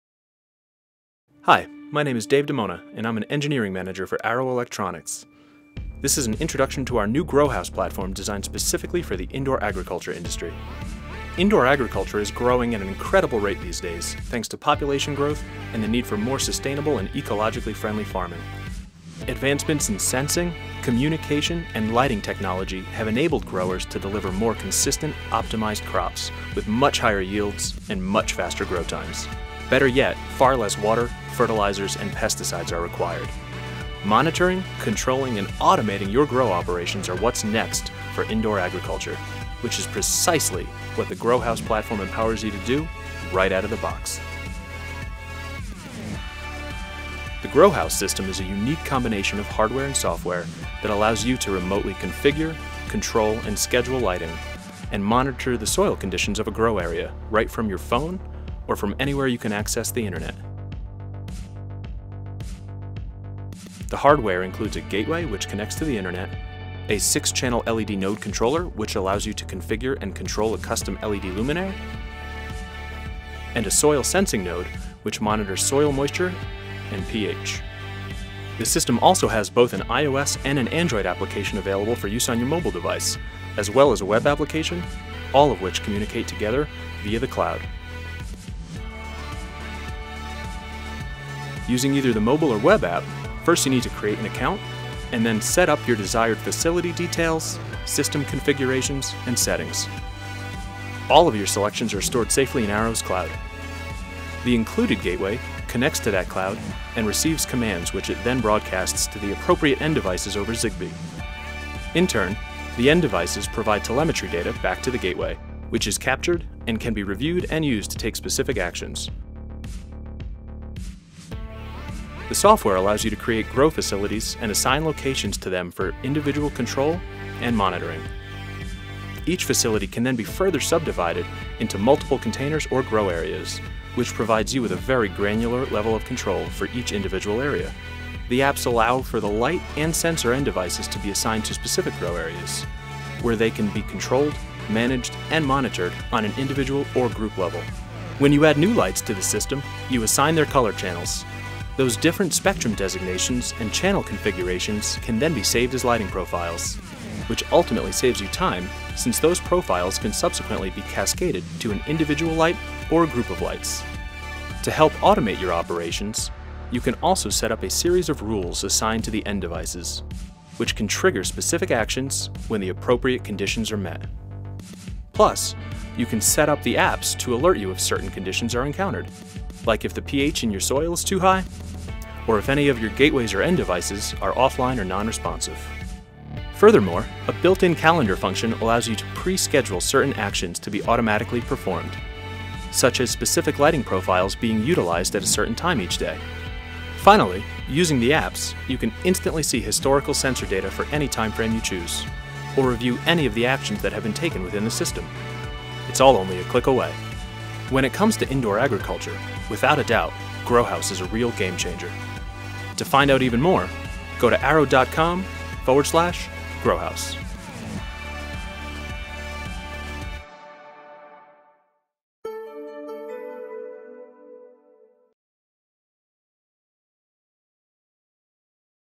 Greenhouse System Overview
Concerns about global population growth, sustainability, and ecologically friendly farming are encouraging growers to adopt innovative technologies to improve overall efficiency. One of the more exciting of these innovations builds upon the Internet of Things (IoT) to enable remote, wireless, real-time sensing, monitoring, and control of indoor farming operations and conditions.

Arrow Electronics has bridged this gap and developed a kit that combines the best-of-the-best technologies, including signal conditioning, converter and power management technology from Analog Devices, wireless connectivity from Silicon Labs, LED dimming and color control of the OSRAM based Light Engine, ruggedized connectors from Amphenol, soil pH and moisture levels and remote accessibility through a cloud-based dashboard.